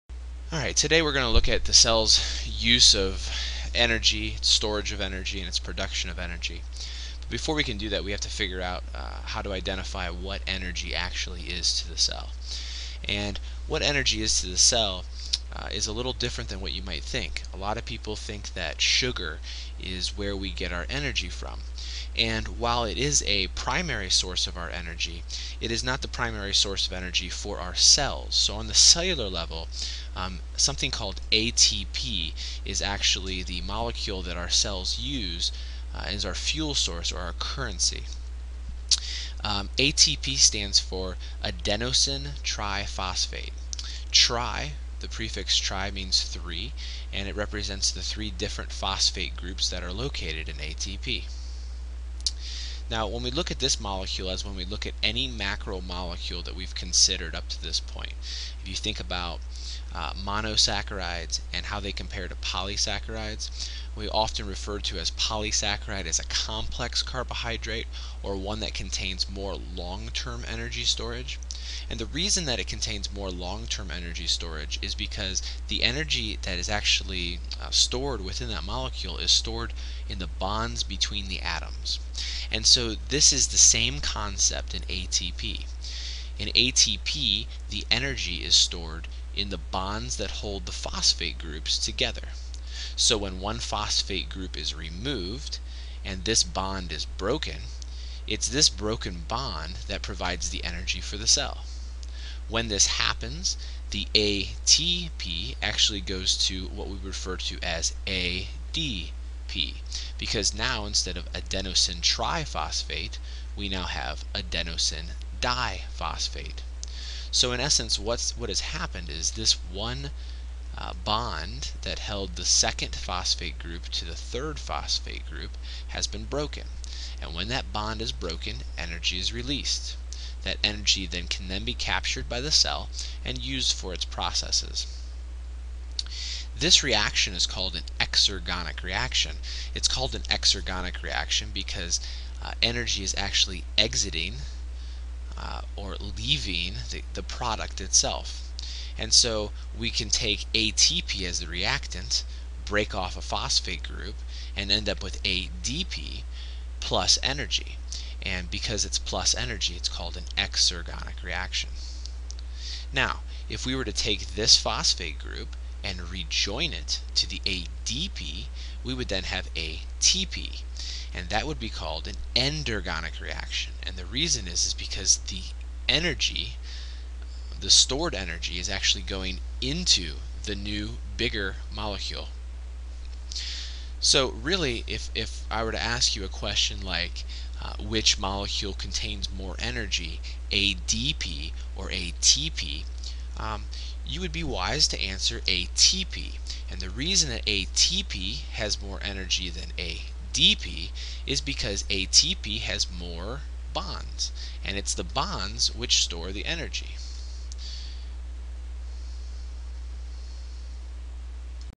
ATP (An explanation of the energy molecule)
ATP is the cell's energy source.  This is a short summary of how it contains energy for the cell to use.